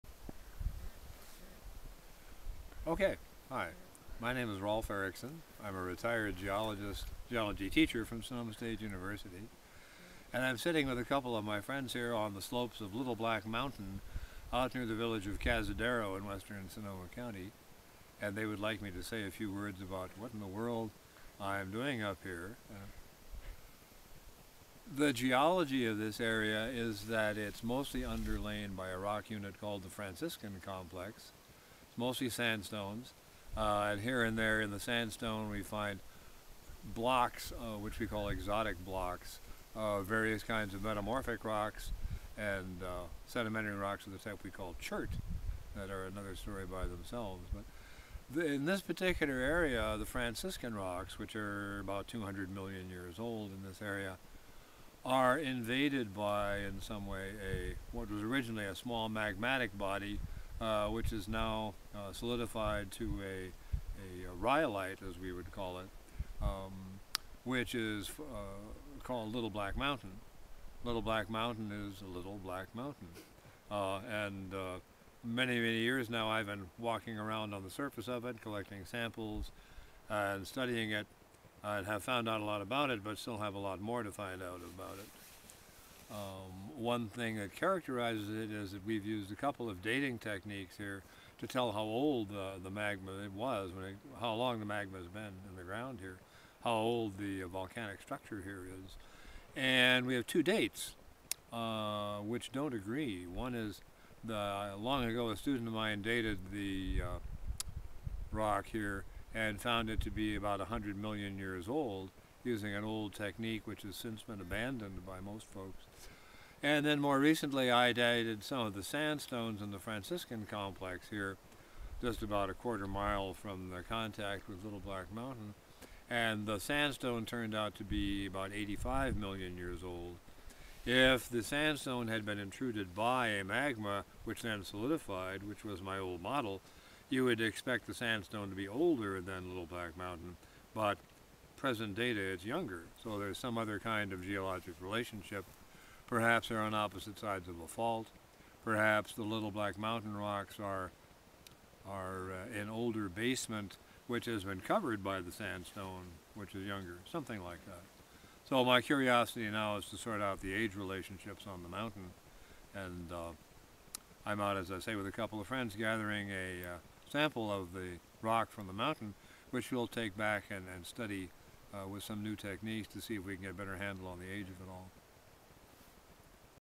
The Geology of Little Black Mountain -- Dr. Rolfe Erickson
Retired Sonoma State University geology professor Dr. Rolfe Erickson and Sonoma Land Trust stewardship staff hiked the slopes of Little Black Mountain in fall, 2010 to collect rock specimens for zircon dating. According to Dr. Erickson, "Little Black Mountain is a small magmatic body of solidified rhyolite that by previous dating methods was considered to be 100 million years old — much younger than surrounding Franciscan metamorphic rocks thought to be about 160 million years old by fossil dates. Recent zircon dating shows the Franciscan to be 83 million years old, and the Little Black Mountain rhyolite date is now too old for the body to be a simple intrusion. We will use the new zircon dates to further resolve this conflict. Another feature of this study is very accurate chemical and mineralogical analysis to characterize this type of rock more precisely." Dr. Erickson has studied the geology of the Cazadero, California area for over four decades and produced multiple papers on the topic.